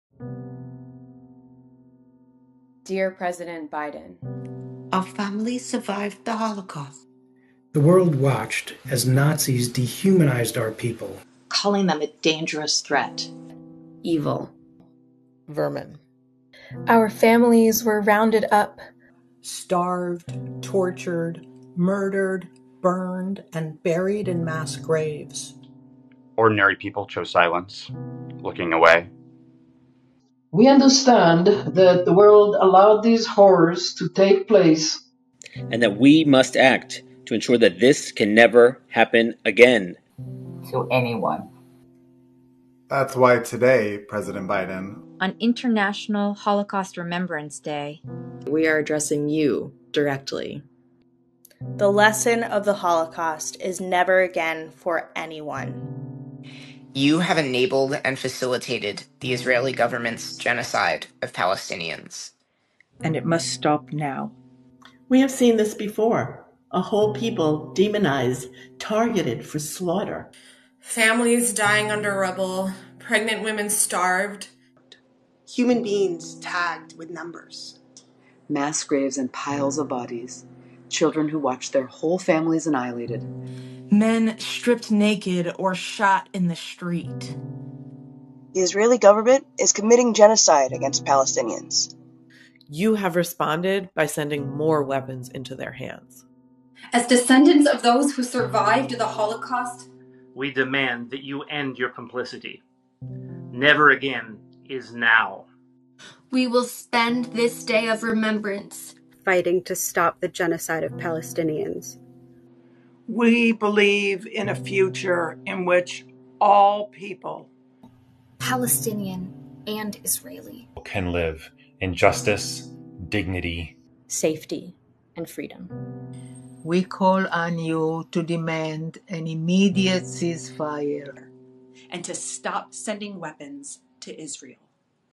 Des survivants de l'Holocauste, appellent le monde à mettre fin au génocide à Gaza
Nous, descendants des survivants de l'Holocauste, appelons le monde en ce Jour de la Commémoration de l'Holocauste à se joindre à nous pour mettre fin au génocide perpétré par le gouvernement israélien contre le peuple palestinien, rendu possible et soutenu par le gouvernement américain.